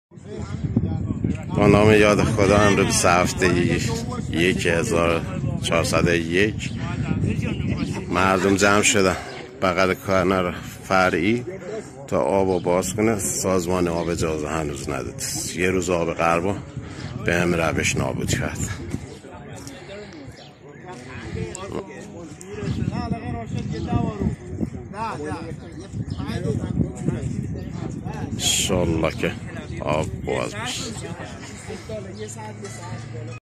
ادامه تجمع کشاوزان اصفهان برای باز کردن آب
روز شنبه ۲۷ فروردین ۱۴۰۱، کشاوزان اصفهان برای باز کردن آب در کنار کانالها تجمع کردند.
لازم به ذکر است که هنوز سازمان آب اجازه باز کردن آب را به کشاورزان نمی دهد.